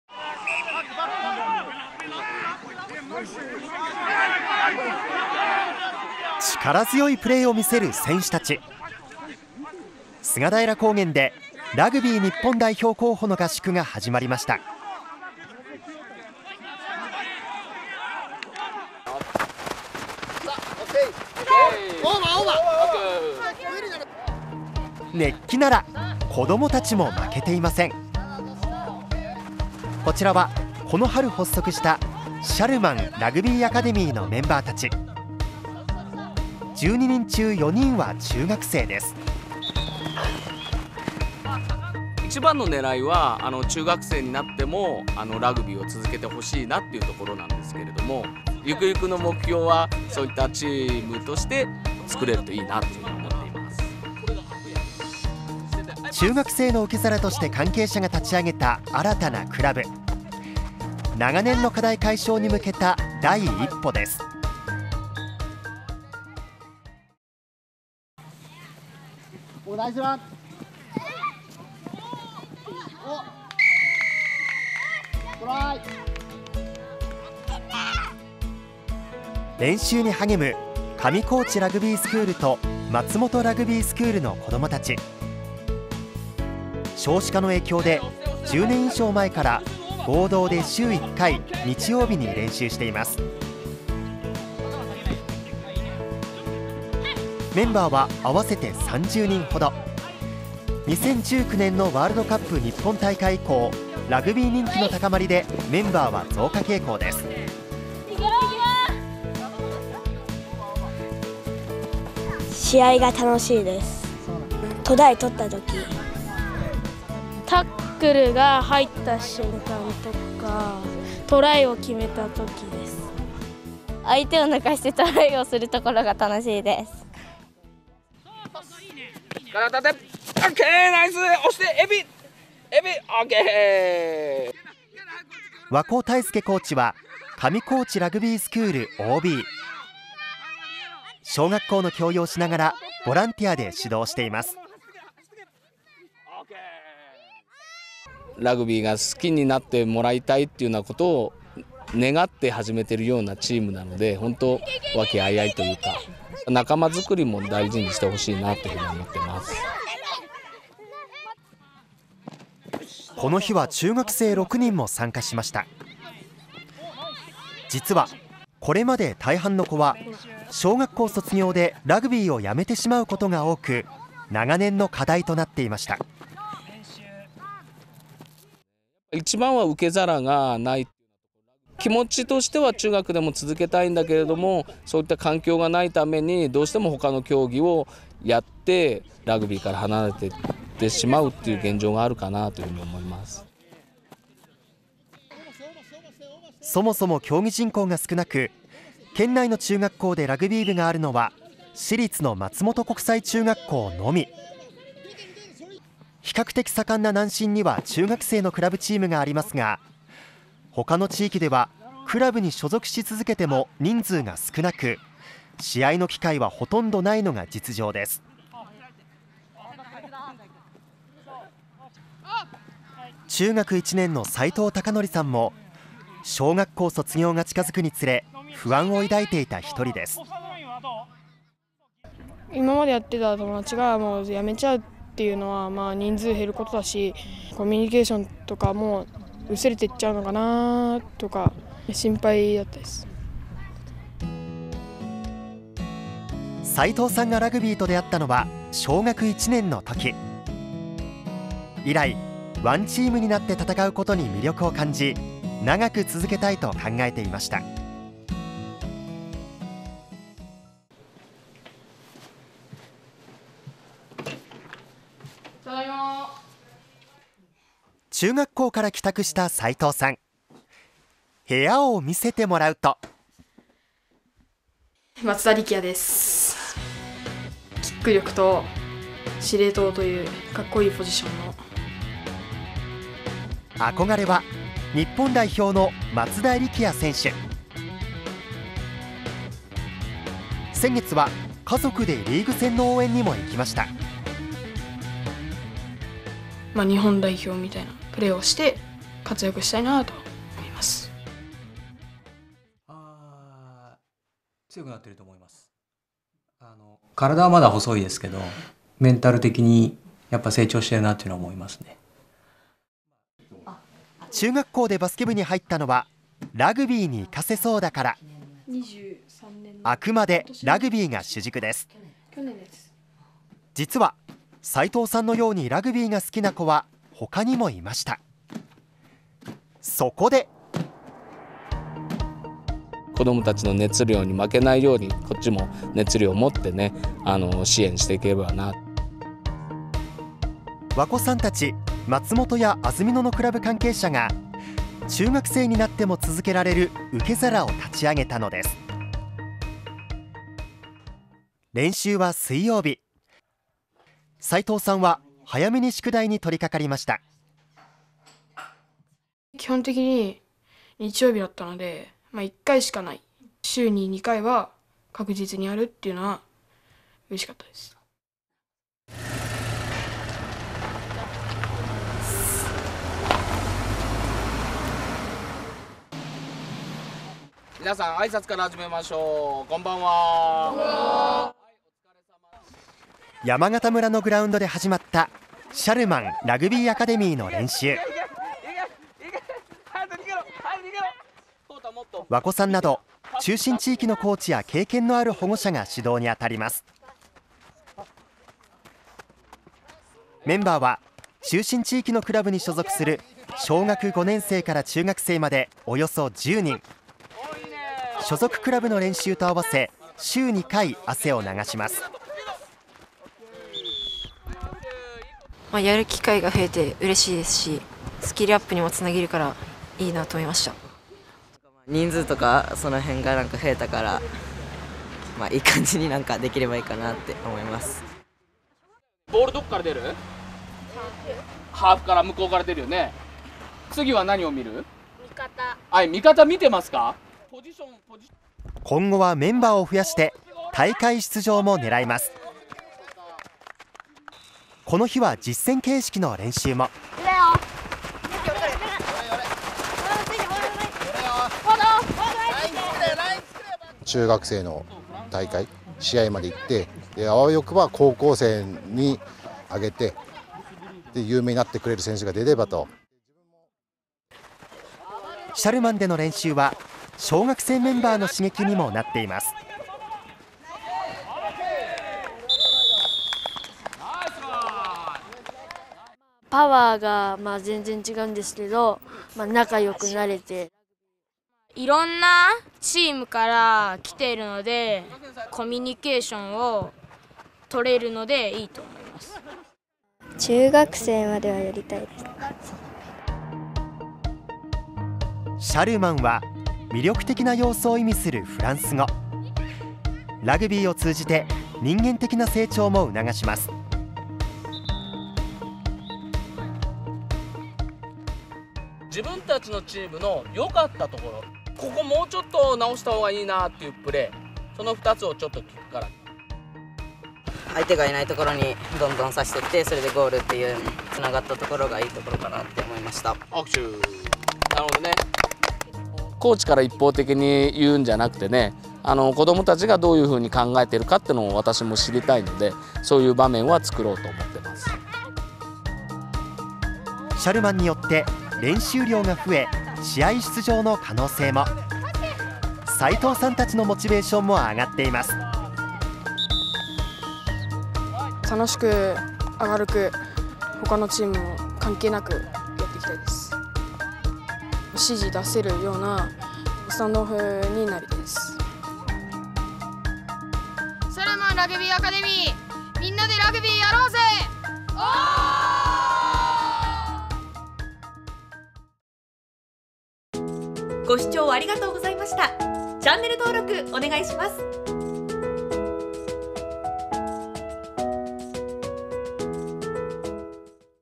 ラグビー日本代表の活躍で人気高まる 県内では部活やクラブ少なく…中学生になってもラグビーやろうぜ！ 中信地域にクラブ発足
特集はラグビーです。日本代表の活躍で近年、人気が高まっていますが、長野県内ではせっかく始めても部活やクラブが少なく、中学生になるとやめてしまうことが多く課題となっていました。そうした中、この春、中信地域で、中学生が競技を続けられる「受け皿」ができ、注目されています。